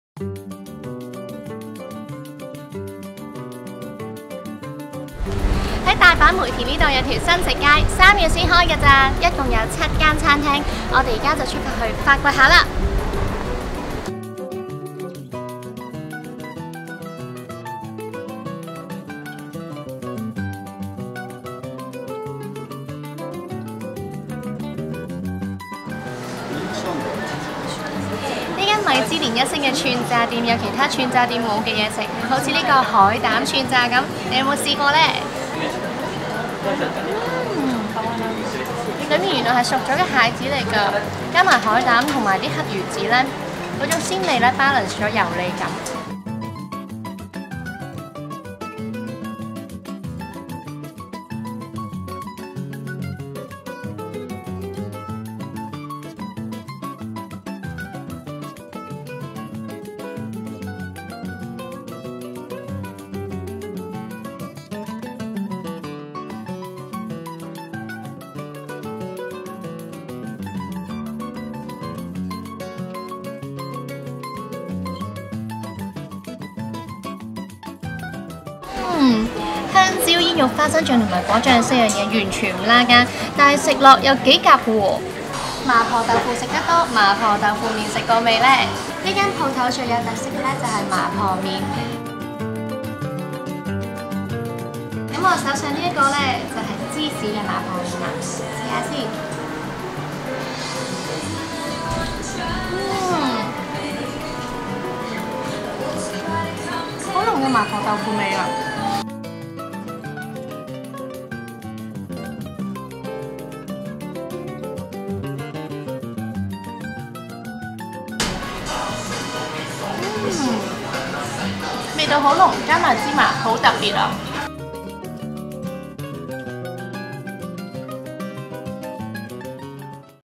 【短綫特搜】大阪新食街3必食 海膽串炸+紐約牛角包、激辛麻婆麵
【大阪必食】講到美食都係去大阪最開心，宜家又多咗條新食街，好掛住嗰間串炸啊～

【info】
茶屋町 Arco
地址：大阪市北區芝田 1-6-2
交通：阪急梅田站茶屋町口直達

串揚げキッチンだん
電話：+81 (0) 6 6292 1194
營業時間：11am - 3pm，5am - 11pm

SHIBIRE-NOODLES 蝋燭屋
電話：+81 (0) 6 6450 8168
營業時間：11am - 10:30pm
全方位緊貼《U Magazine》社交平台